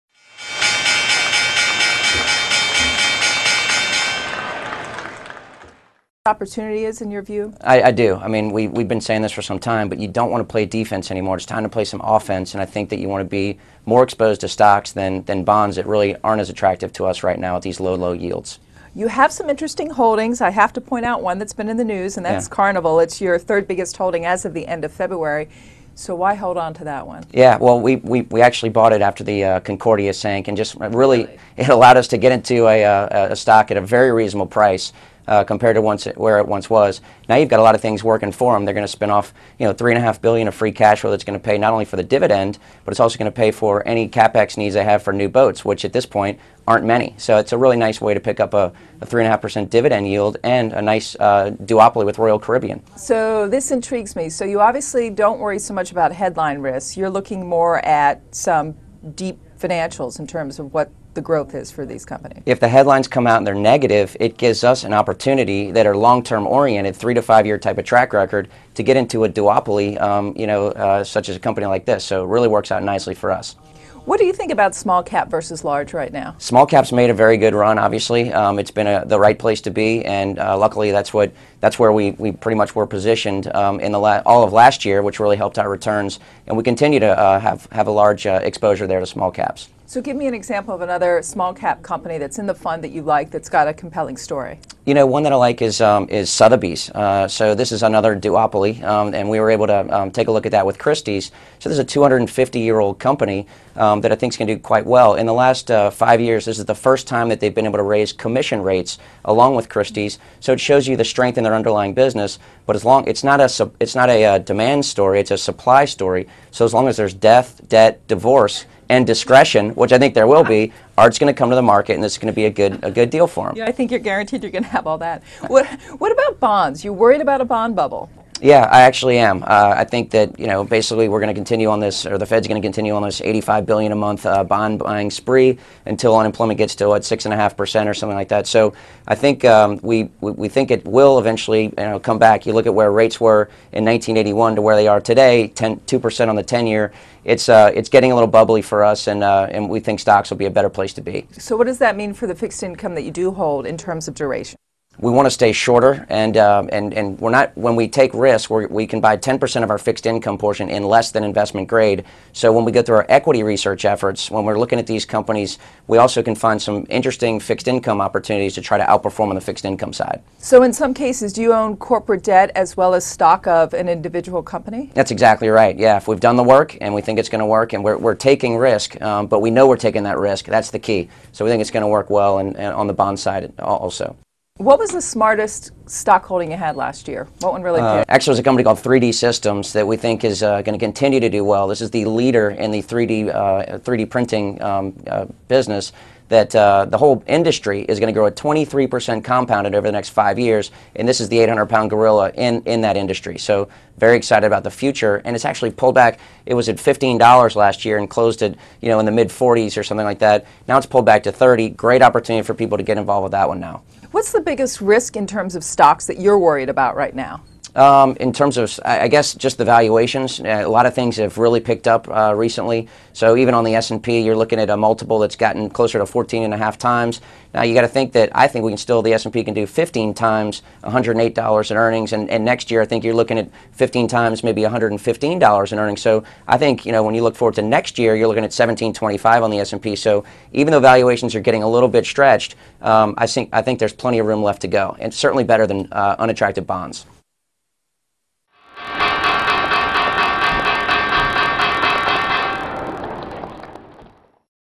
Now's the time for investors to take risk
ANCHOR (OFF-CAMERA) ENGLISH SAYING:
Now you manage a balanced fund, about two-thirds right now in stocks. Is that where the best opportunity is in your view?
 SANDY VILLERE, MANAGER, VILLERE BALANCED FUND (ENGLISH) SAYING:
I do. I mean, we've been saying this for some time but you don't want to play defense anymore. It's time to play some offense and I think that you want to be more exposed to stocks than bonds that really aren't as attractive to us right now at these low, low yields.
You have some interesting holdings. I have to point out one that's been in the news and that's Carnival. It's your third biggest holding as of the end of February so why hold on to that one?
Yeah, well, we actually bought it after the Concordia sank and just really- ReallyIIt allowed us to get into a stock at a very reasonable price compared to where it once was. Now you've got a lot of things working for them. They're going to spin off $3.5 billion of free cash flow that's going to pay not only for the dividend but it's also going to pay for any capex news they have for new boats, which at this point aren't many. So it's a really nice way to pick up a 3.5% dividend yield and a nice duopoly with Royal Caribbean.
So this intrigues me. So you obviously don't worry so much about headline risk, you're looking more at some deep financials in terms of what the growth is for these companies.
If the headlines come out and they're negative, it gives us an opportunity that are long-term oriented, three to five-year type of track record to get into a duopoly such as a company like this so it really works that nicely for us.
What do you think about small cap versus large right now?
Small caps made a ...